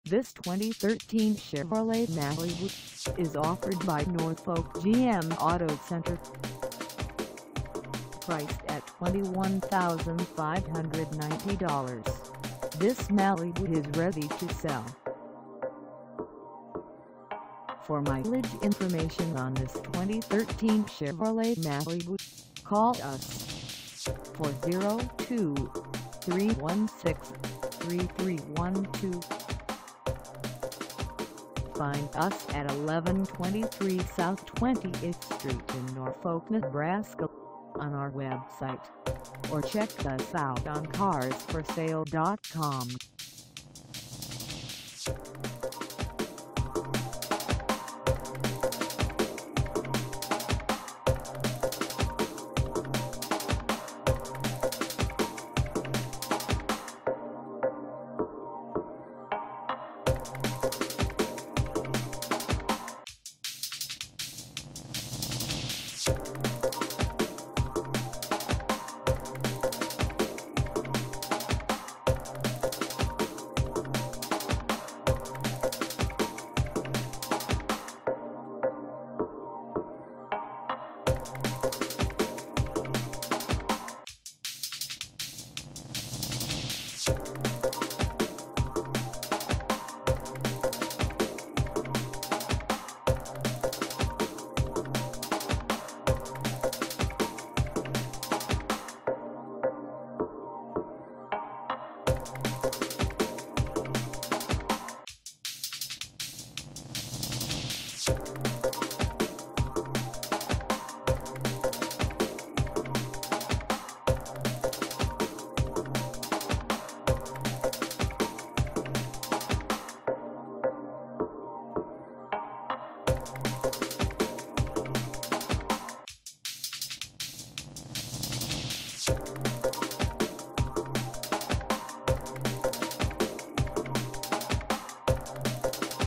2013 Chevrolet Malibu LT - for sale in Norfolk, NE 68701
Come test drive this 2013 Chevrolet Malibu LT for sale in Norfolk, NE.

Air Conditioning,Anti-Lock Brakes,Automatic Transmission,Front Wheel Drive,Power Brakes,Power Steering,Power Windows,Tilt Steering Wheel